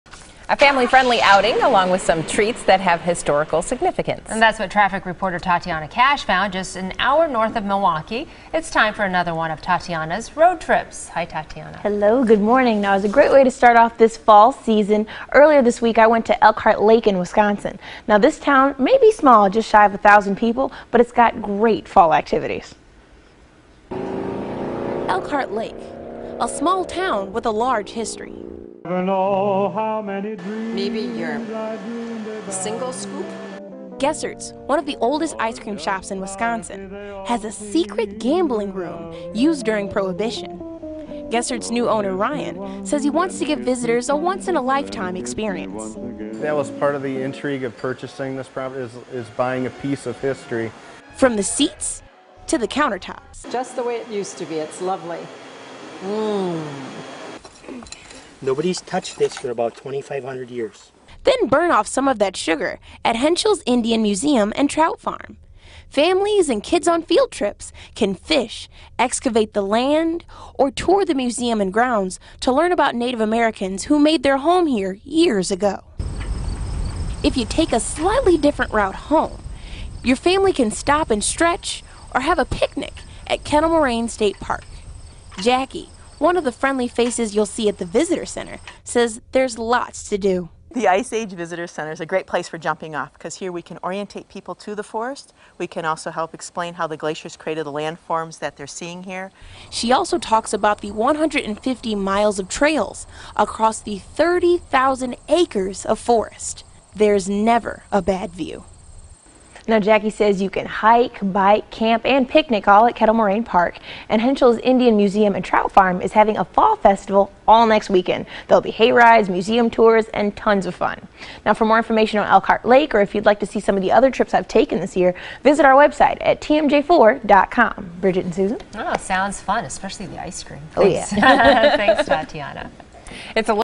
TaTiana's Road Trips: Fun in Elkhart Lake
TaTiana takes a road trip to Elkhart Lake.